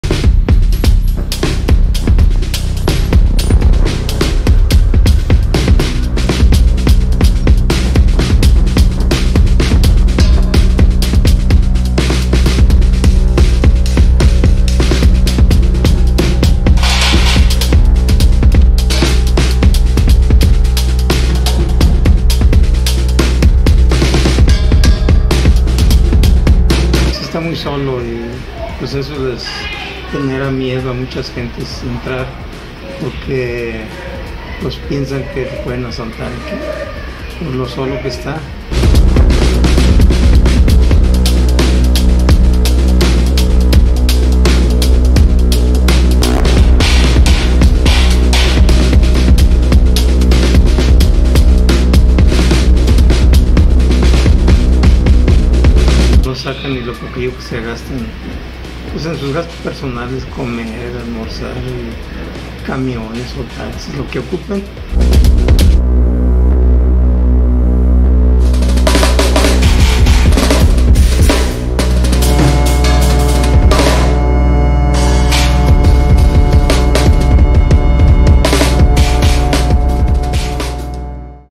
Cierran locales del Mercado de Artesanías de León por falta de ventas
Poco a poco se ha ido extinguiendo su esencia del comercio de artesanías y la marroquinería ____________________________________________